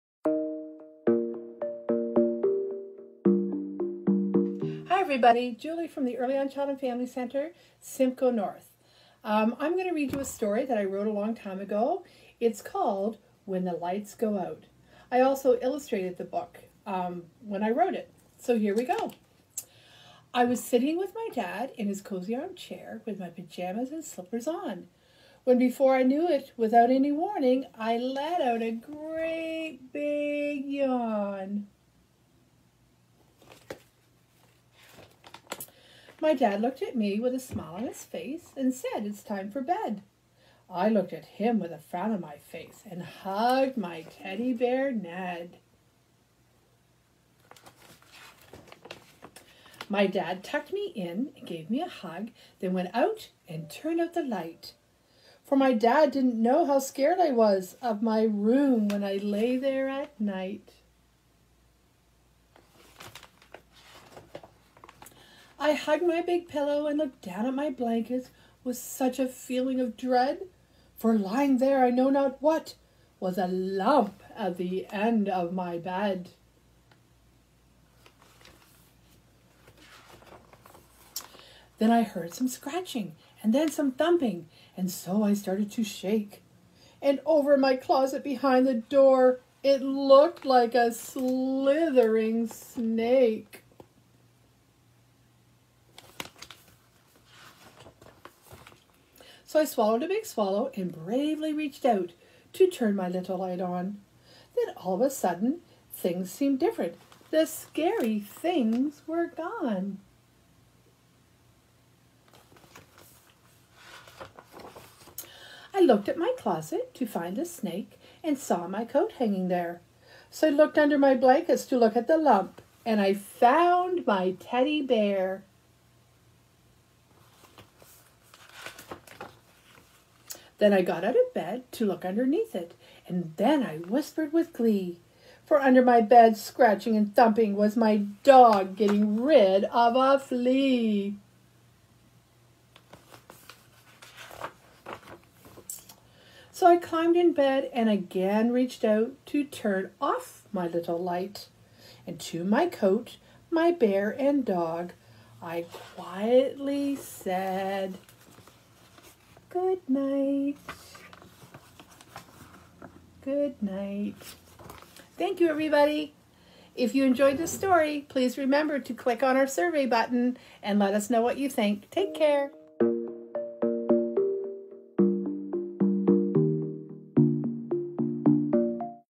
Story Time with Julie
Join Julie as she reads a story she wrote years ago called When the Lights Go Out!

Please let us know how we are doing by filling out this brief survey.